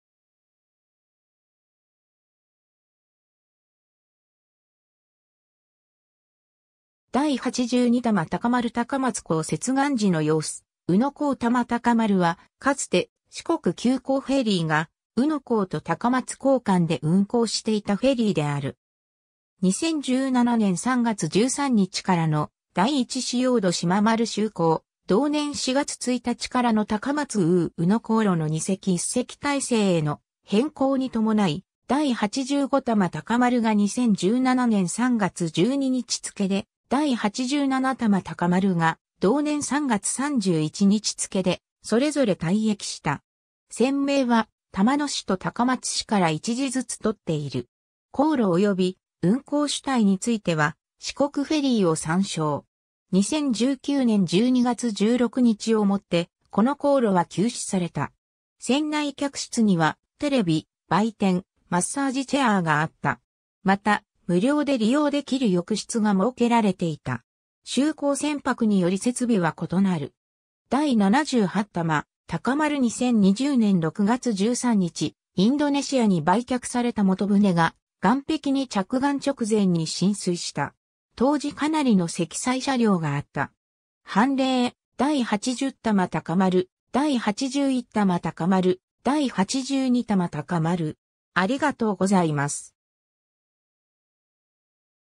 玉高丸
第八十二玉高丸 - 高松港 接岸時の様子（第八十二玉高丸） - 宇野港 玉高丸（たまたかまる）は、かつて四国急行フェリーが宇野港と高松港間（高松・宇野航路）で運航していたフェリーである。
 2017年3月13日からの「第一しょうどしま丸」（高松ー土庄航路から転籍）就航、同年4月1日からの高松ー宇野航路の2隻→1隻体制への変更に伴い、第八十五玉高丸が2017年3月12日付で、第八十七玉高丸が同年3月31日付で、それぞれ退役した。
 船名は、玉野市と高松市から1字ずつ取っている。
 ※航路および運航主体については、四国フェリーを参照。
 ※2019年12月16日をもってこの航路は休止された。
 船内客室には、テレビ・売店・マッサージチェアーがあった。
また、無料で利用できる浴室が設けられていた。
 ※就航船舶により設備は異なる。
 第78玉高丸（右） 2020年6月13日、インドネシアに売却された本船(現船名Dharma Rucitra 3)が岸壁に着岸直前に浸水した。
当時かなりの積載車輛があった。
 ※凡例：(80) - 第八十玉高丸、(81) - 第八十一玉高丸、(82) - 第八十二玉高丸